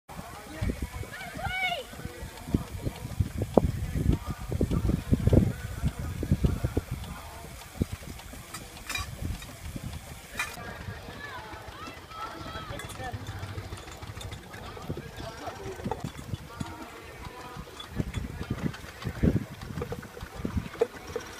4" McLaren 'Murphy' Ticking Over
A short video of our old 4" McLaren road locomotive 'Murphy' ticking over at the 2014 Cornish Steam & Country Fair held at Stithians Showground.